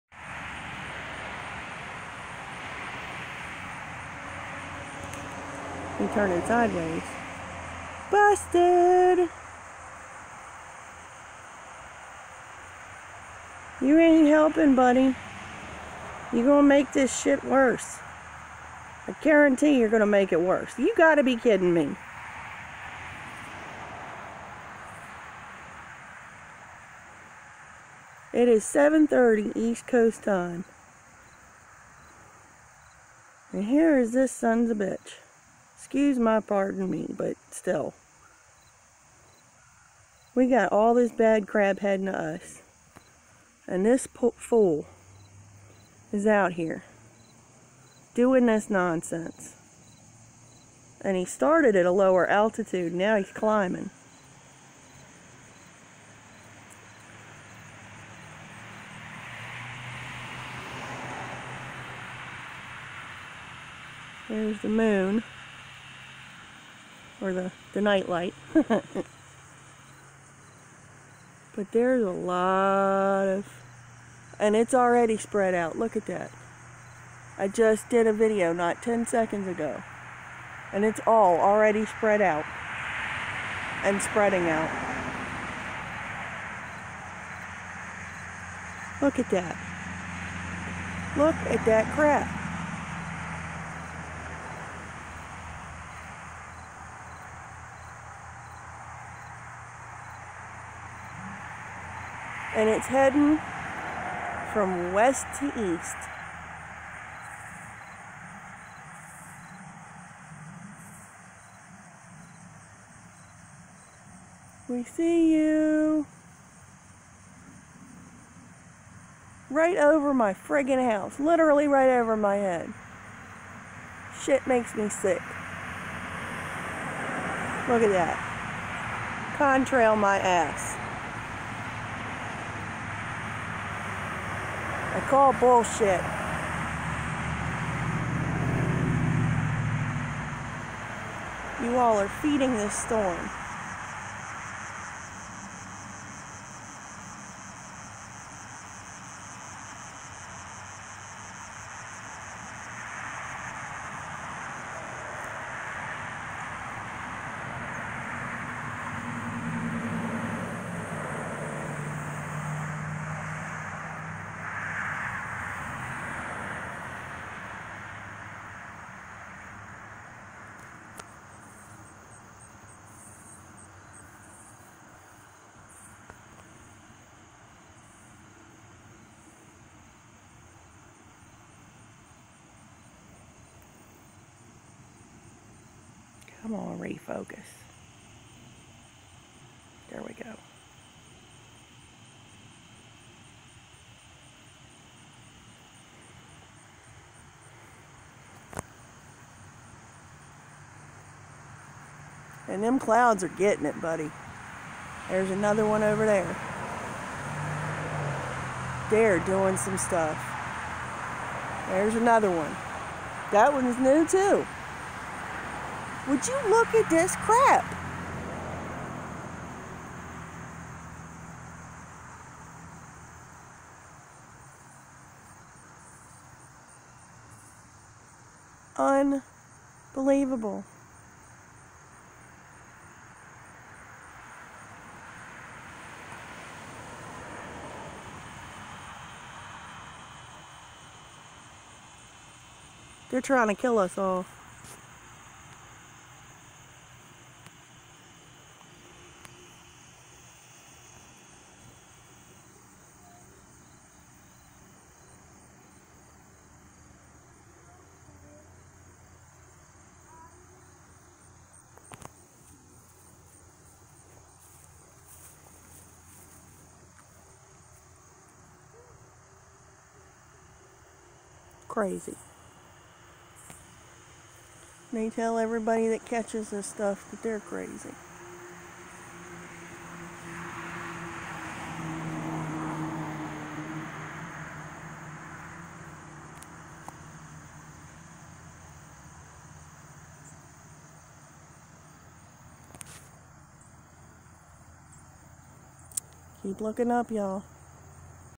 Creating a monster???
At 7:35 est tonight many seed trails were spotted at once...